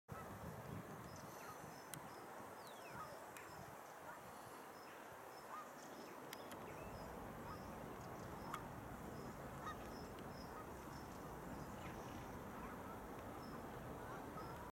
Little Egret, Rochdale Canal SSSI, Greater Manchester
Little Egret on Boarshaw Lane Flash, adjacent to the Rochdale Canal Site of Special Scientific Interest, Greater Manchester, filmed on 30th October 2020